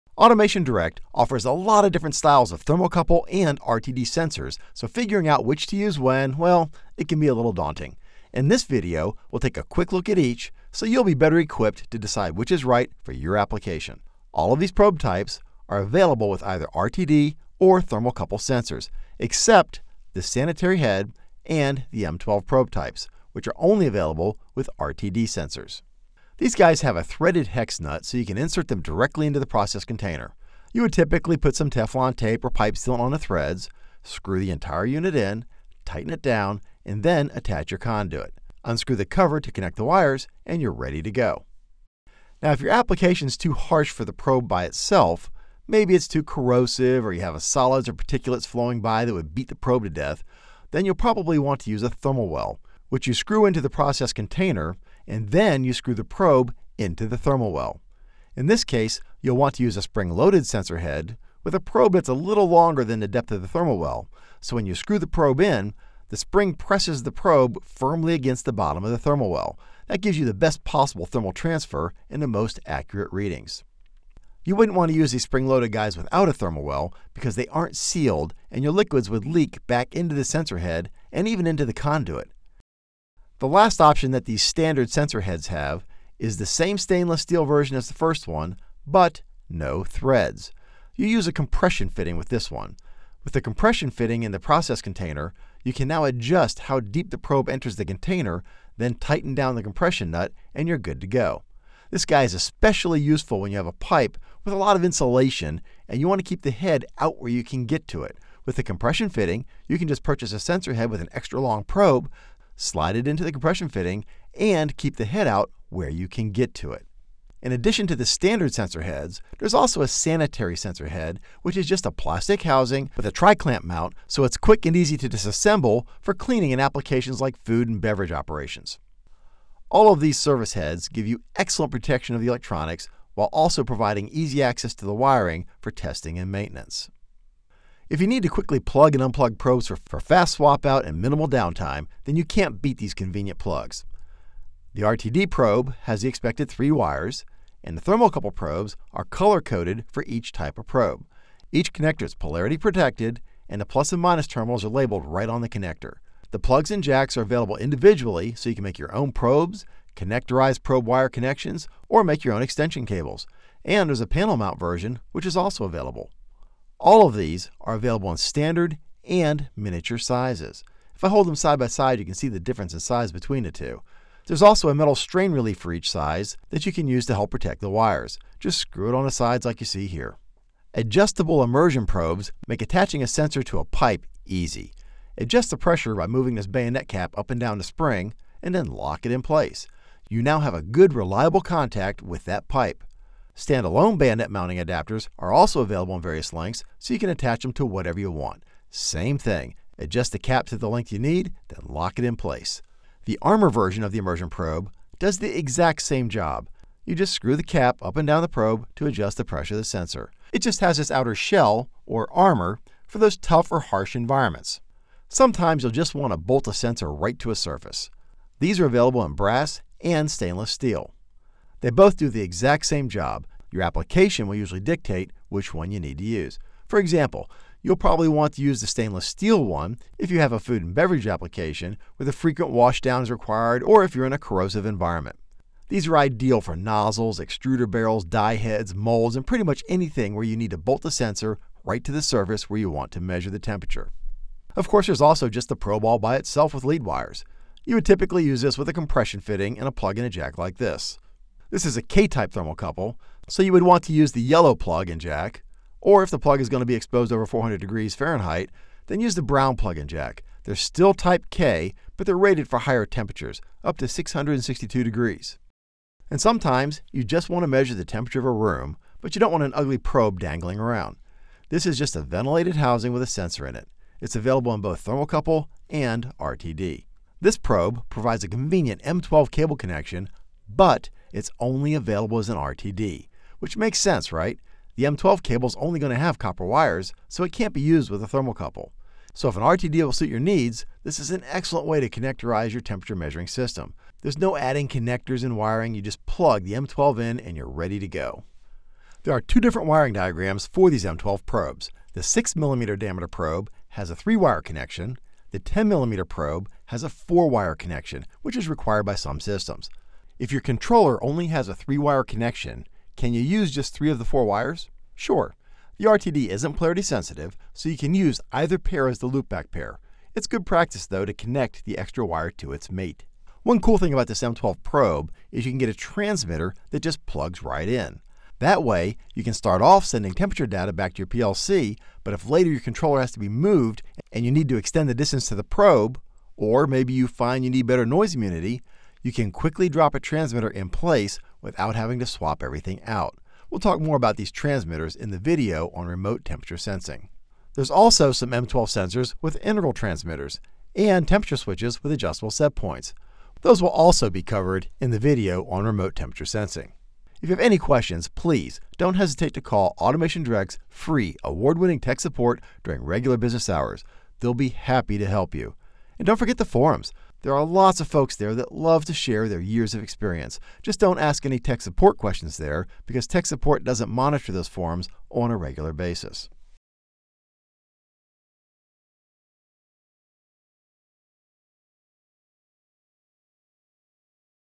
Essential Guide to Industrial Temperature Probes at AutomationDirect
Learn about all the different types of temperature probes and which ones will work best for your application..